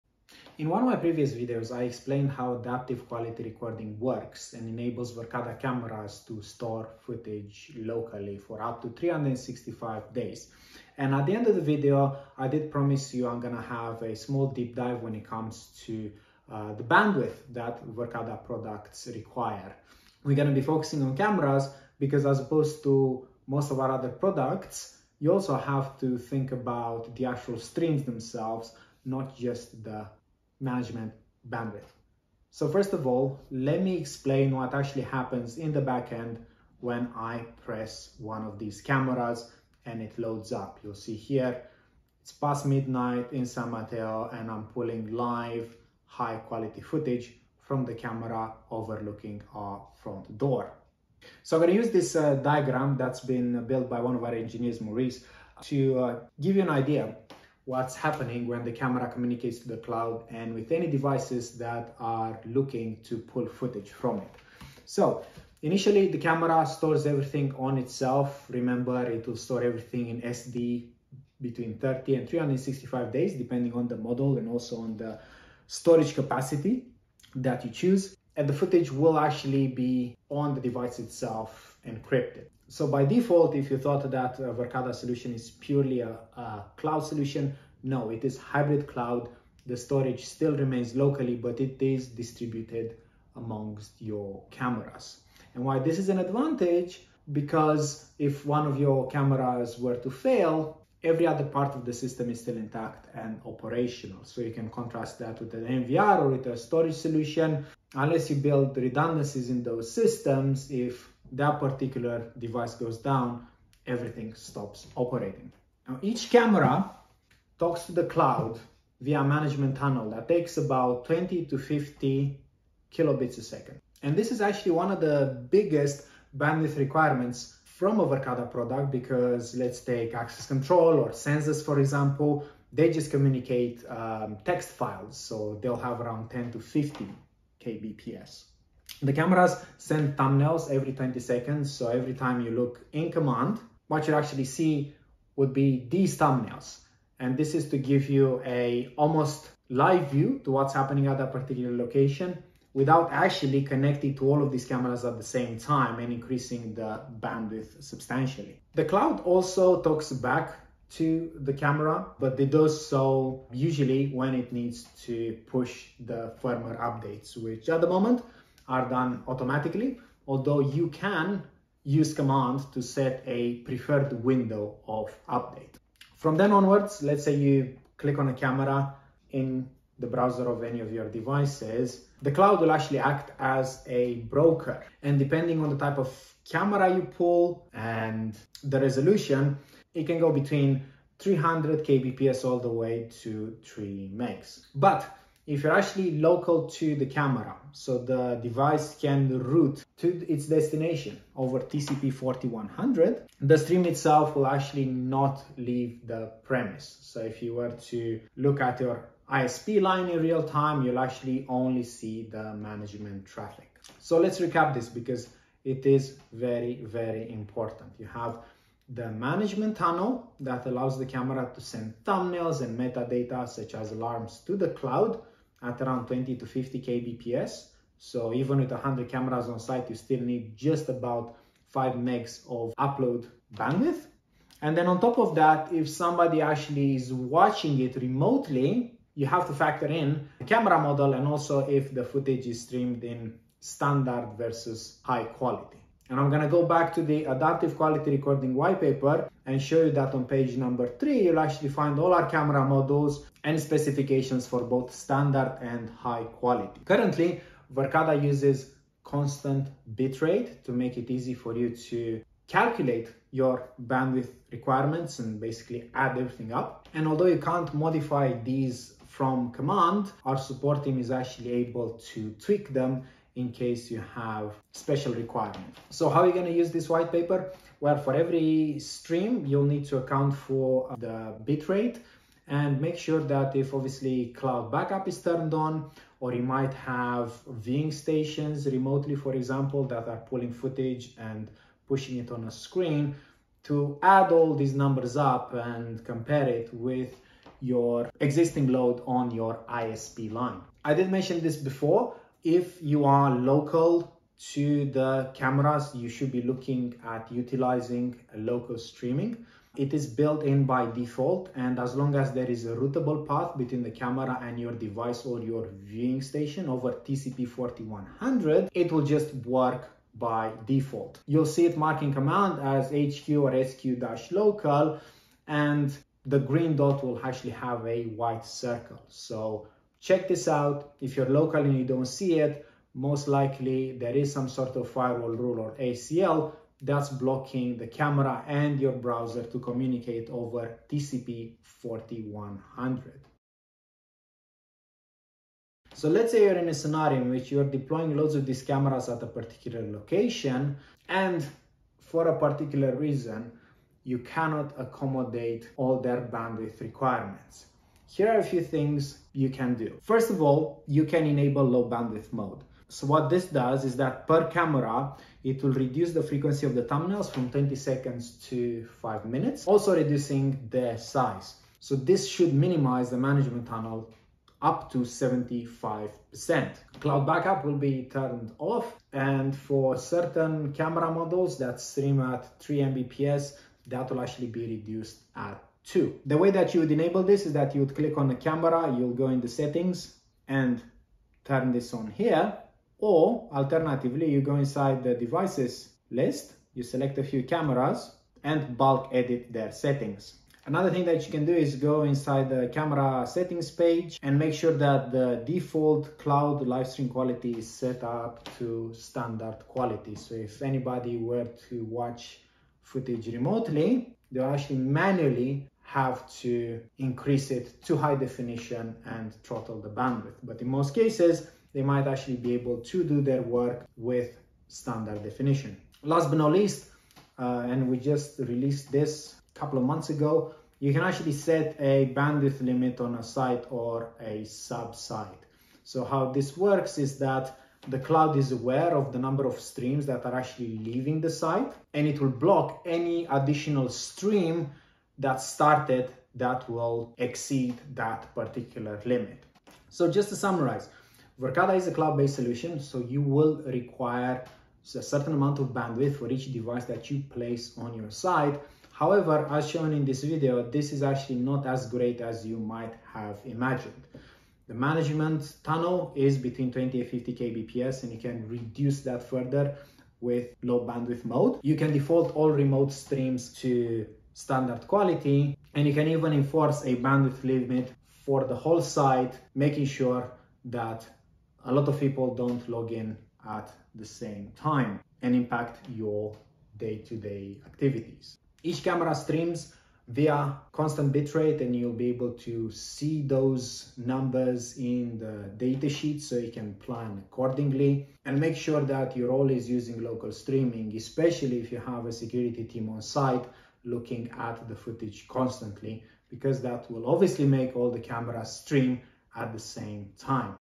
Verkada - Bandwidth Considerations
Any Verkada solution requires access to the Internet, however it is very bandwidth friendly. To visualize this better, I will focus on cameras, as it is the most complex product in this perspective. After an in-depth explanation on the required bandwidth, I will highlight a few ways to reduce it further, in case you are struggling with WAN availability.

00:00 - Camera streaming process explained
04:16 - The use of CBR & bandwidth calculations
05:00 - Local Streaming
05:43 - Tips for sites with low bandwidth
07:29 - Summary

🗒️ Verkada camera bandwidth:

➡️ Made out of a management tunnel (approx. 20-50Kbps per camera, upstream), and any remotely initiated streams or backups

➡️ Local Steaming refers to the ability of the cameras to stream locally, directly to the device viewing them, and will work as long as there is a routable path and TCP4100 is not blocked

➡️ Streaming is done with Constant Bit Rate, to help you more easily calculate your ISP requirements

➡️ Protect your ISP line further by defaulting streams to SQ, and enabling a bandwidth limit per site

🔗 Adaptive Quality Recording whitepaper (see page 3 for steaming bandwidth requirements per camera)